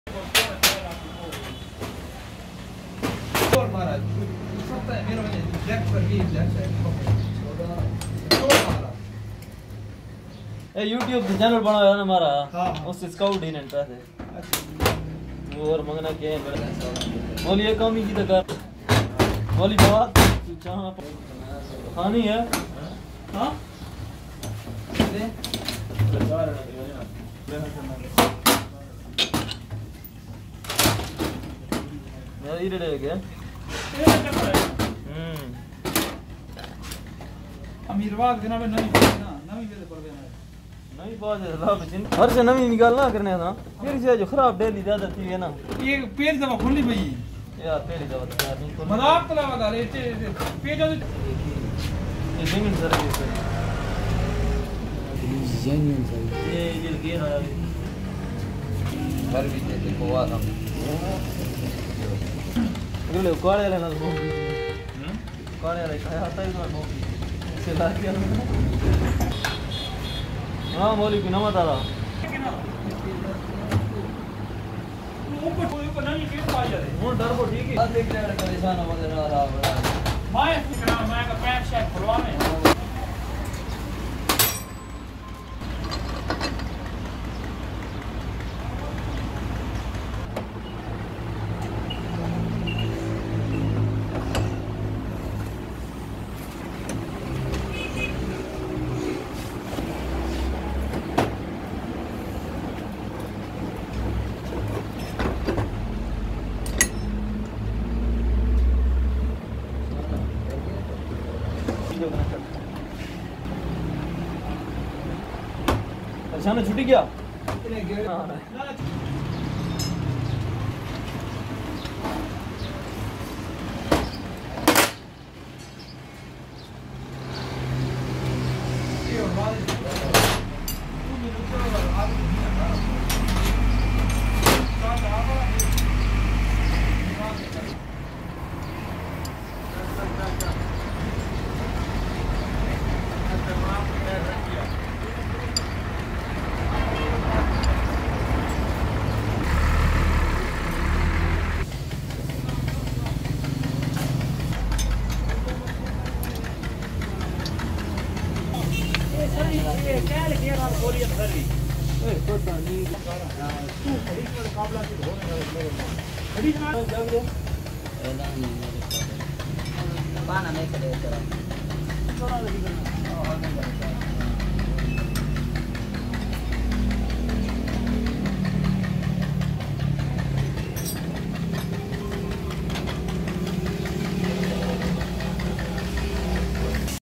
How to Replace Defective Clutch Servo | Short Clip of Replacing Clutch Booster
Welcome to Pakistani Trucks.
My Goal is to provide truck repairing, truck review and other Interesting and Informational videos about truck.